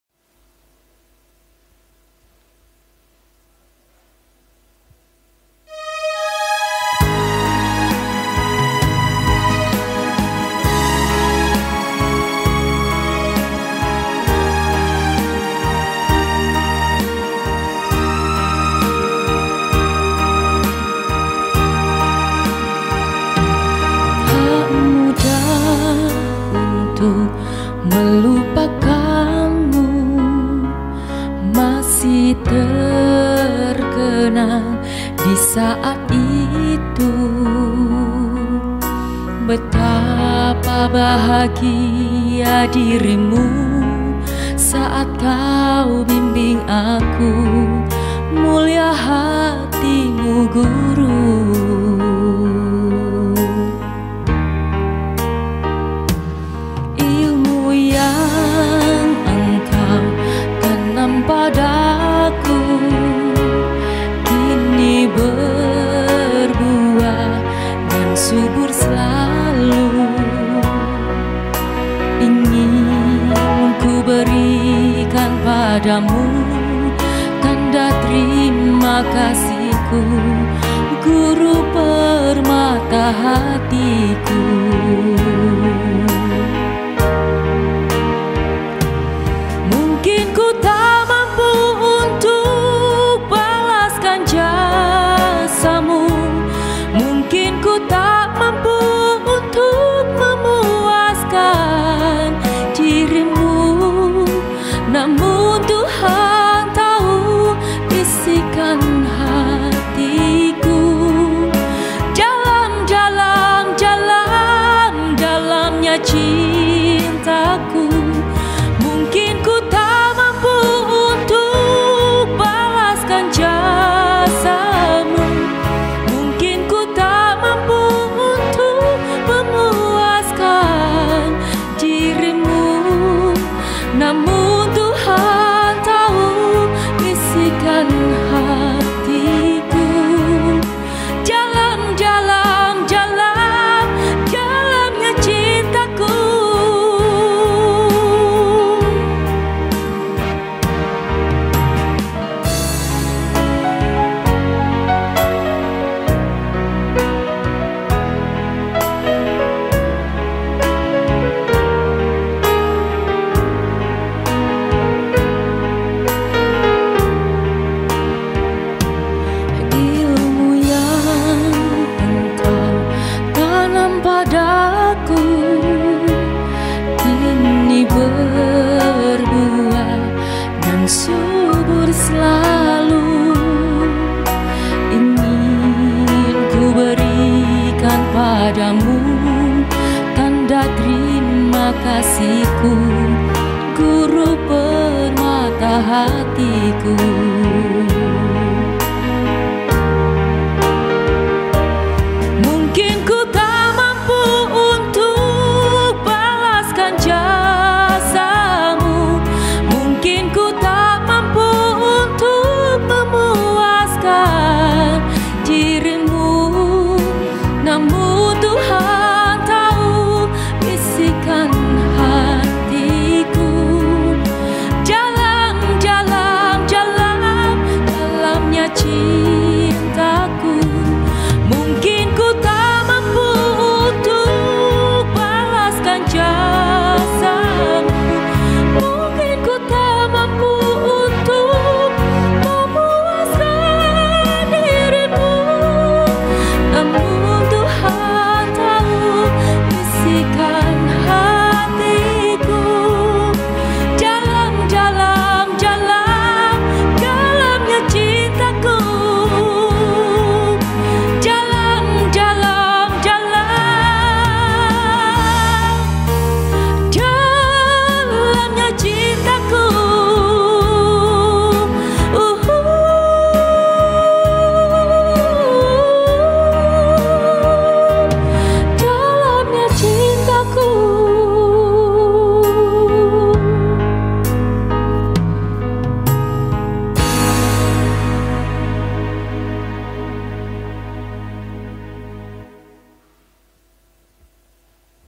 LOMBA SOLO VOCAL PGRI JATENG: ELLY MAHARANI SHAKTI (Kota Salatiga)
Peserta Lomba Solo Vocal dalam Rangka HUT ke-76 PGRI dan HGN Tahun 2021 Tingkat Provinsi Jawa Tengah

Nama Peserta:
ELLY MAHARANI SHAKTI
SMP N 3 Salatiga

Asal Peserta:
PGRI Kota Salatiga